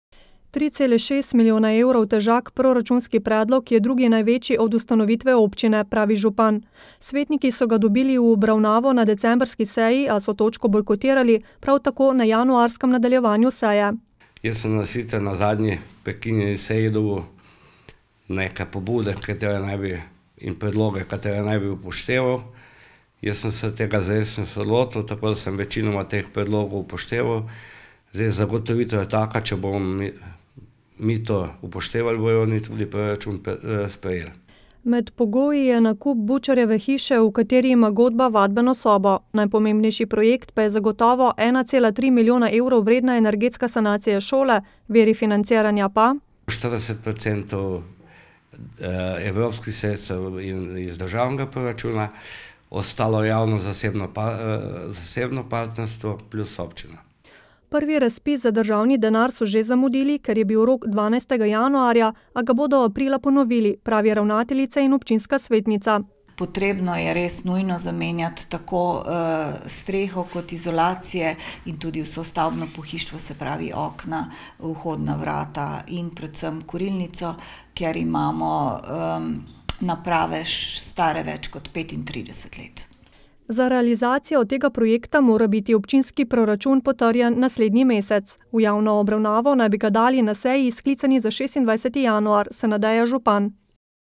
Nujna energetska sanacija šole
KOSTANJEVICA NA KRKI - Potem ko so lani v svetniški skupini bojkotirali sprejetje proračuna, se v Kostanjevici na Krki bojijo podobne zgodbe tudi letos. V proračunu je namreč predvidena energetska sanacija tamkajšnje osnovne šole, na nujnost katere opozarjajo že vsaj pet let, saj za energente porabijo okrog 65 tisoč evrov letno. Z blokado obravnave proračuna pa je ta projekt ogrožen.